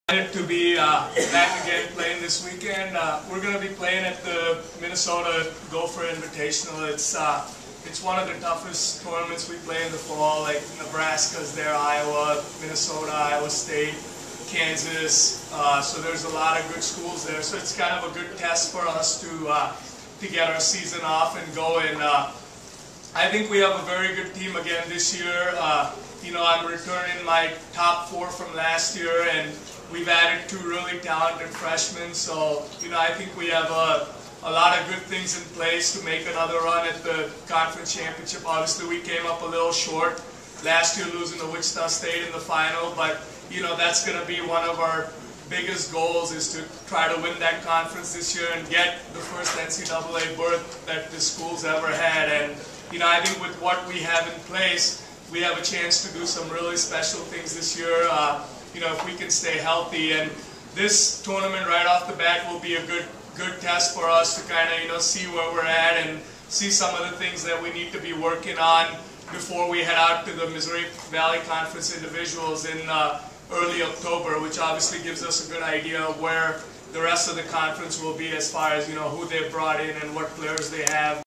UNI Women's Tennis (Sept. 21, 2009)
Sachin Kirtane, head women's tennis coach, reviews the upcoming schedule for the Panthers' tennis team.